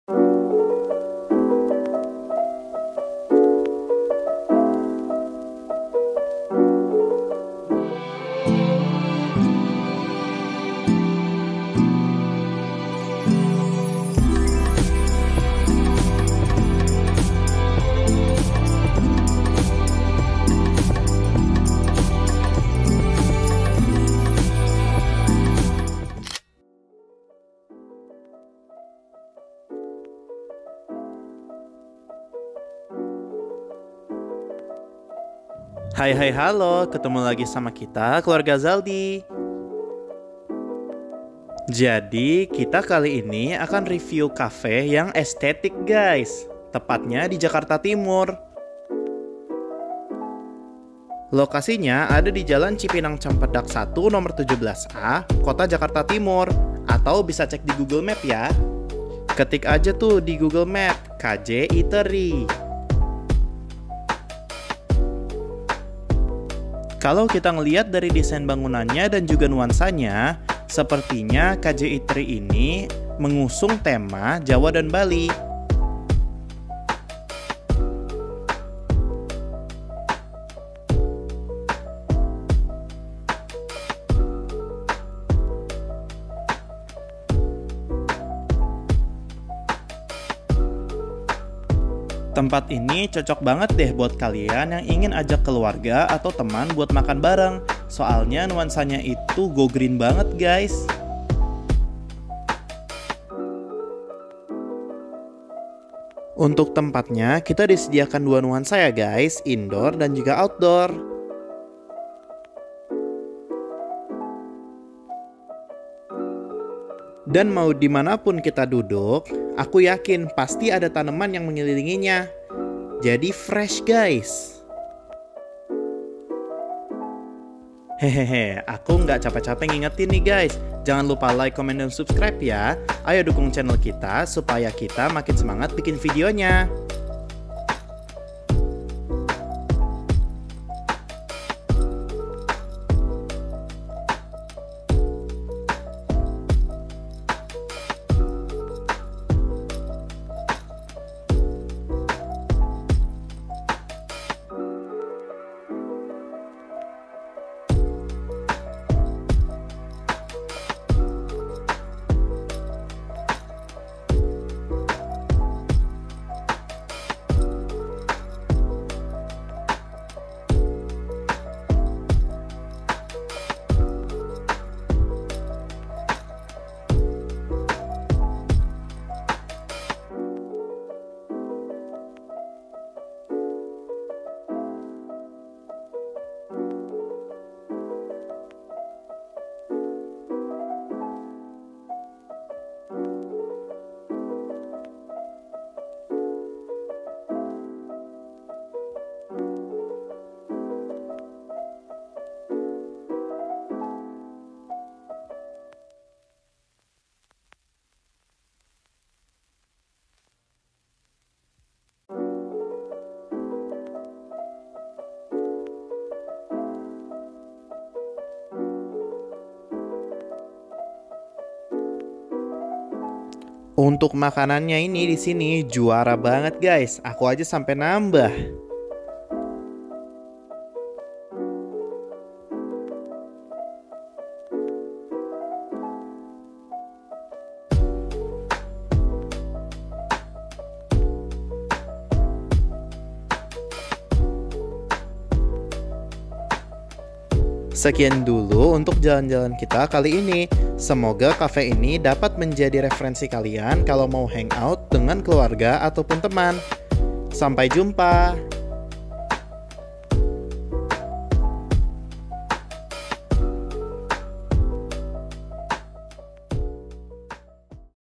Kaje Eatery & Coffee || Kuliner Jakarta Timur || Kaje Cafe Aesthetic
Haloooo kita lanjut jalan jalan virtual bareng Keluarga Zaldy yukksss. Kali ini kita mau ngajak ke cafe yang aesthetic di daerah Jakarta Timur loh. Ikutin kita yaaa